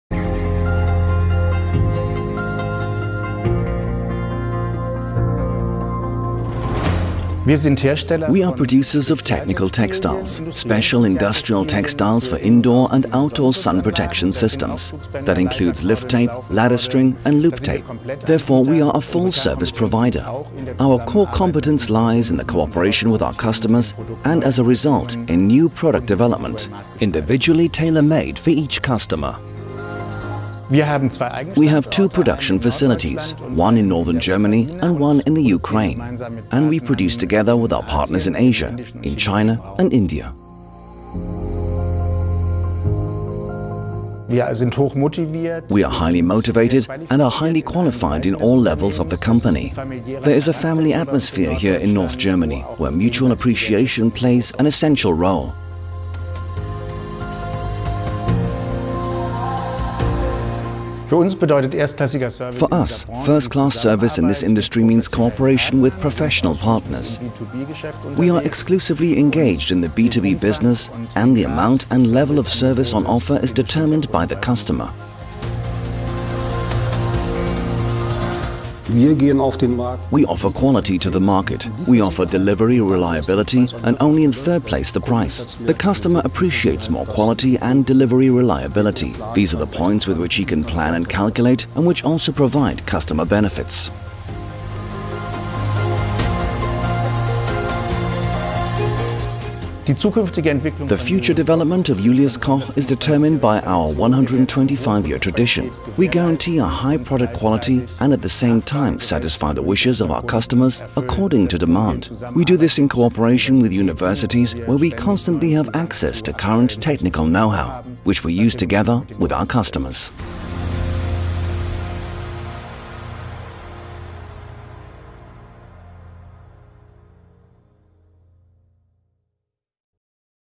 JULIUS KOCH - We keep it together. (ENG)
At Julius Koch, we develop and produce textile solutions for the global market, such as cords, tapes, ladder strings, threads and nets to the highest technical standards.Our experience in the field of highly specialized textile solutions as well as our continuously growing know-how uphold Julius Koch’s foremost expertise in manufacturing textile innovations and cost-efficient products that offer one thing above all: maximum performance for exceptional requirements. Our production facilities are located in Europe: in Kreuzfeld, Germany and Lviv, Ukraine. But our scope of activity is global. And our production output covers the whole world: Each year, we manufacture about 175,000 miles of top-quality specialty textiles, which, wrapped around the equator, would span the planet seven times. Our employees in offices in Germany, Denmark, Italy, Ukraine and China are the first port of call for customers from all over the world.